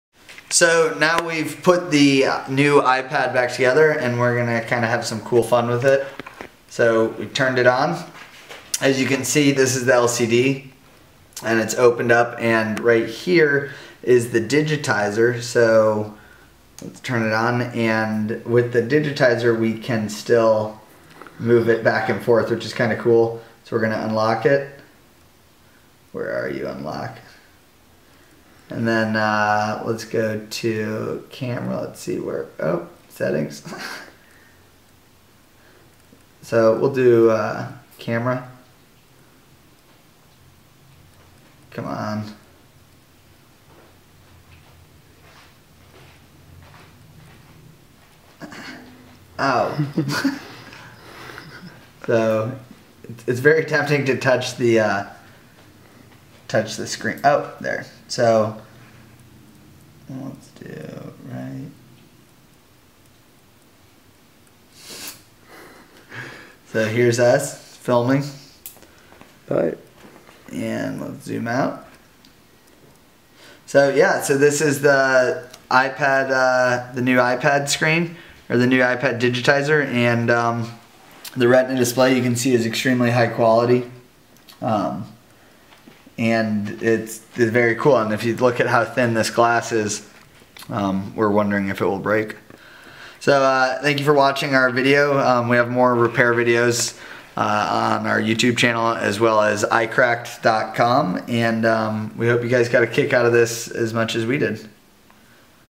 The iPad 3 / New iPad Disassembled and Functioning
- On March 16, 2012, the release of the iPad 3 / New iPad, iCracked performed an official teardown of the iPad 3 / New iPad the same day it came out to see how it looks on the inside. As you can see, the digitizer still works even when not in contact with the LCD.

iCracked will of course will be supplying replacement parts and DIY Kits to repair any broken iPad screen, LCD, or battery in the near future!

iPhone, iPod, and iPad are trademarks of Apple Inc.